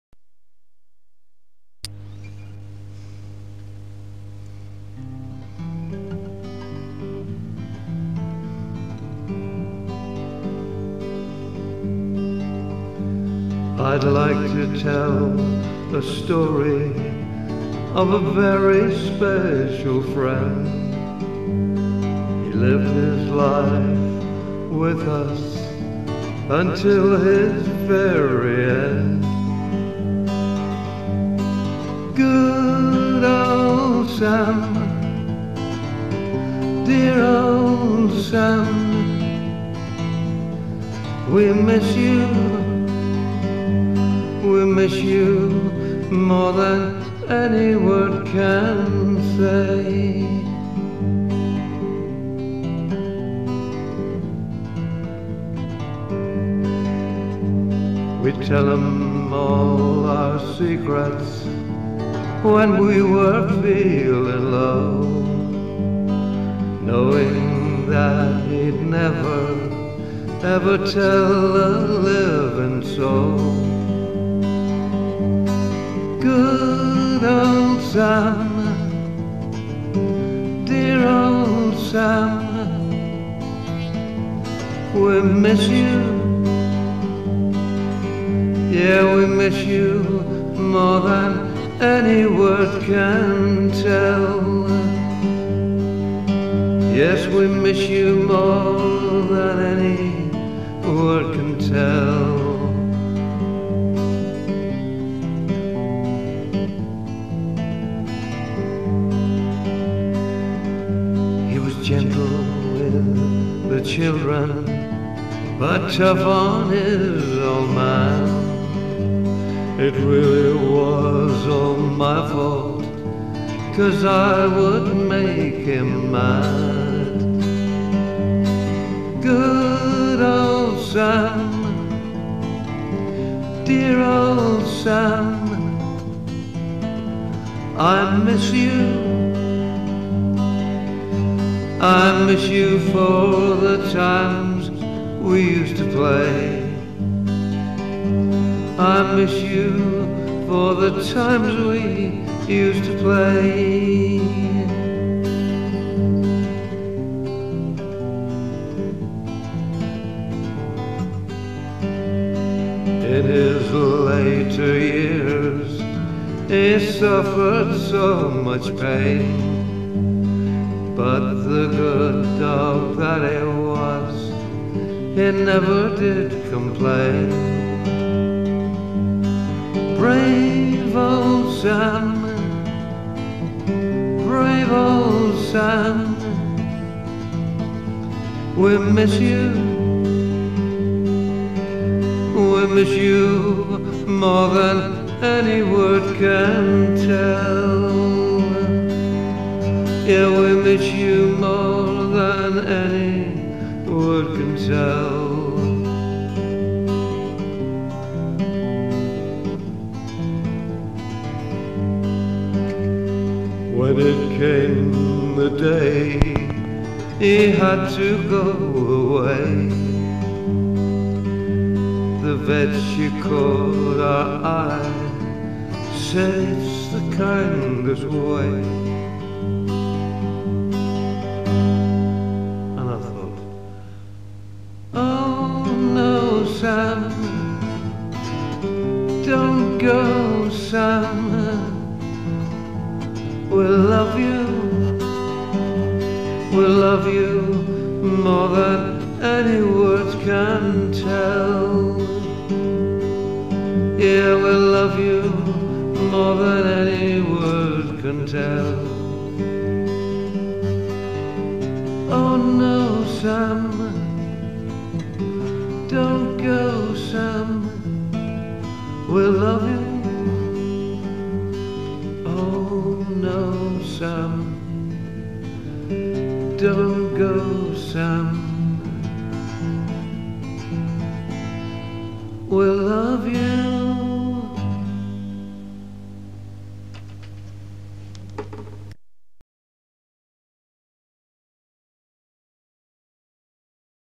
Tribute to our dog Sam
Our Border Collie,Sam,was with us from a puppy until he sadly passed away at the age of 17, leaving a huge gap in our lives. He was a big dog but gentle as a lamb with children,other animals and us, taking on a caring role for anyone he thought needed him and his protection. This song was composed as an antidote to grief.. We will always miss him .